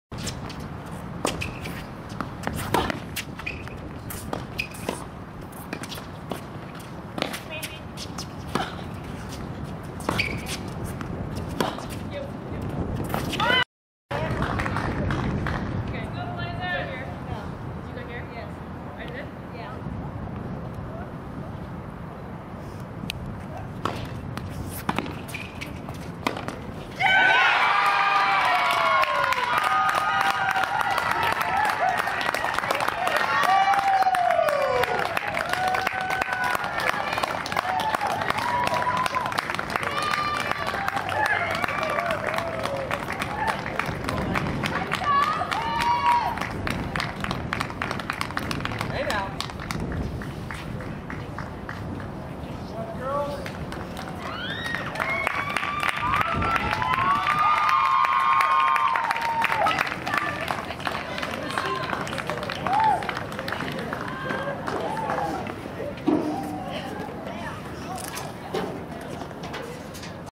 OHSAA Ohio Girls Tennis - Division II Doubles - 2016 - OhioTennisZone.com
Hathaway Brown's Ally Persky and Catherine Areklett defeat Gilmour Academy's Claudia Althans and Carly Cohen.